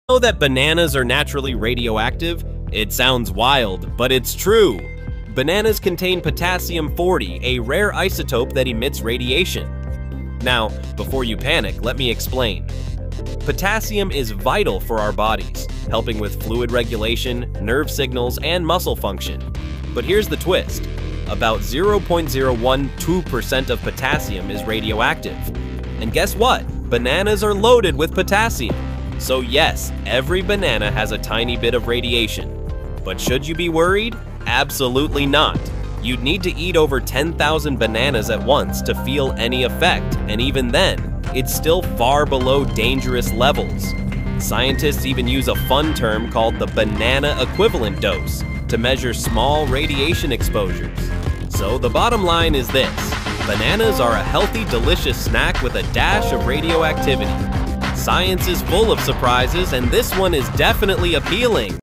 Banana 🍌 Are Radioactive?! The Shocking Truth!
Bananas are more than just a delicious fruit 🍌—they’re slightly radioactive! 😮 But what does that really mean? In this video, we uncover the science behind bananas’ radioactivity, explain potassium-40, and discuss why this fascinating fact is nothing to worry about. Discover the quirky side of science with us!

Got a topic in mind? Drop your ideas in the comments, and let’s explore together!